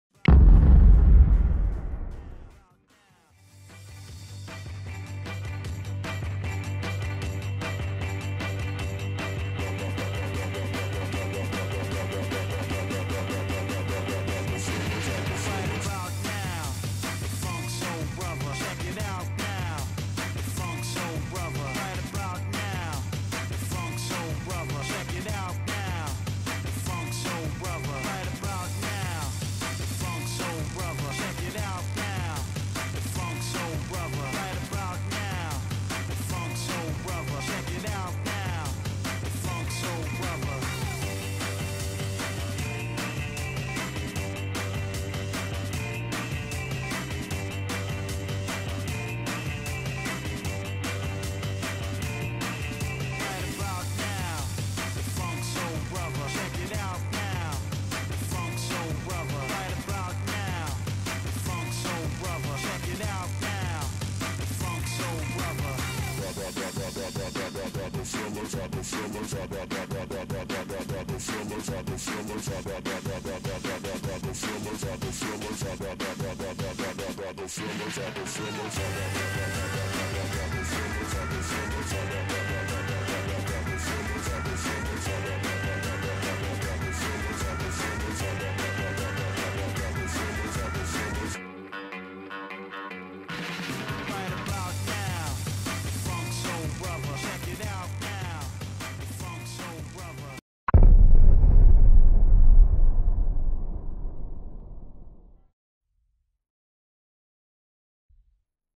Dobbi - Chasing
My chihuahua chasing its favourite 5 year old stuffed toy.